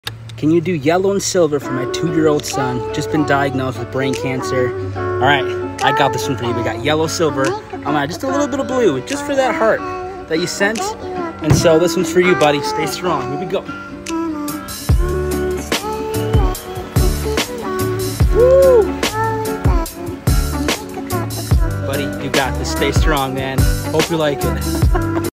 Spin Art Stories! Son Diagnosed With Cancer😢
Spin Art Stories! I turn tragic stories into art with paint and a drill!

SEND ME MAIL:
511 E Walnut Street